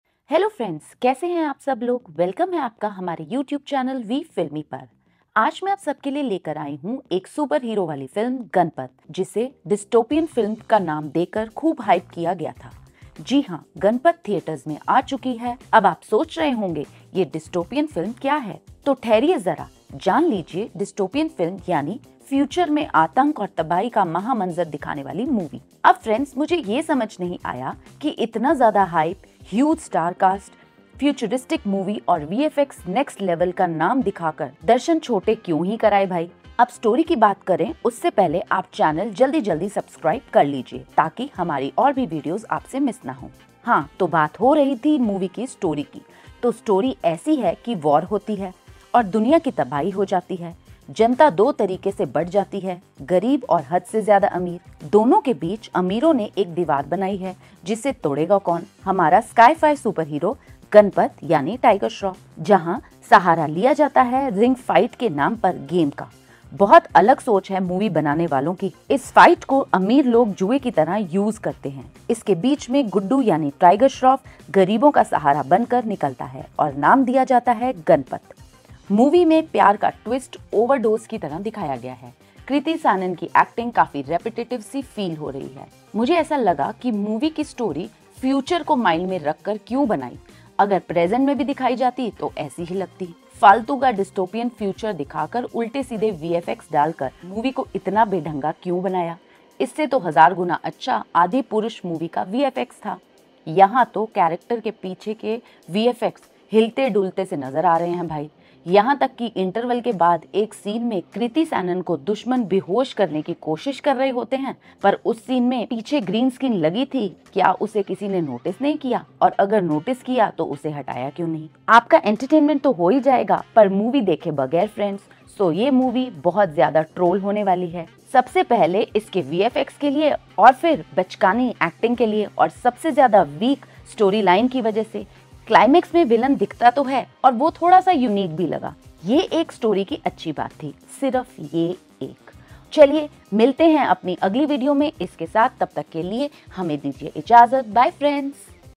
Ganapath Movie Review Tiger Shroff's Dual Role Fails to Impress (2023)
In this video, I will review Ganapath and give my honest opinion on its story, direction, acting, music, action, and visual effects. If you are a fan of Tiger Shroff or action movies, you might want to watch this video before you decide to watch Ganapath.